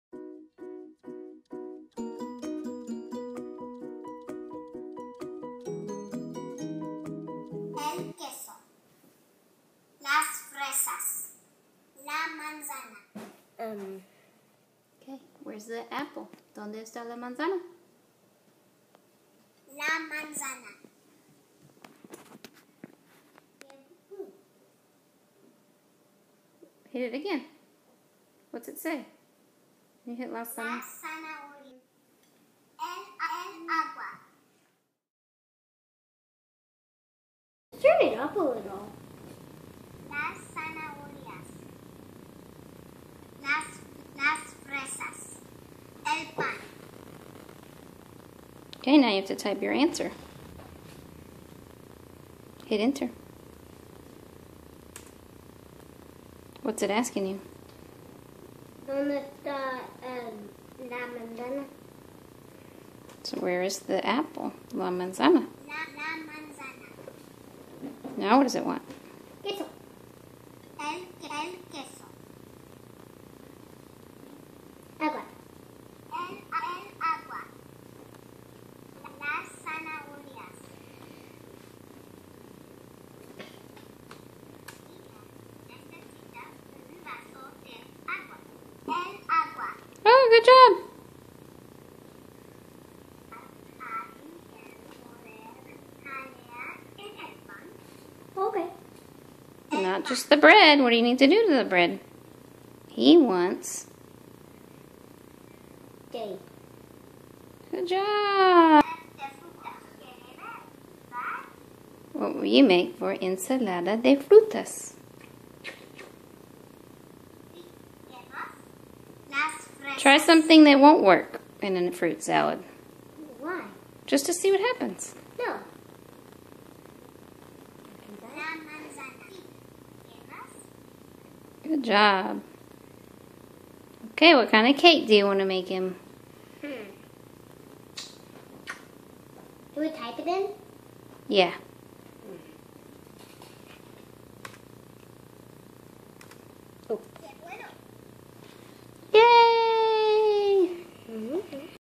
Spanish Game for Makey Makey
This is a video of the Spanish Game created for Makey Makey Spanish Game Lesson.  This Spanish game is to be played with real objects by utilizing a MaKey MaKey to connect the correct keys to the objects. The purpose of this video is to show how students will interact with real objects while practicing Spanish!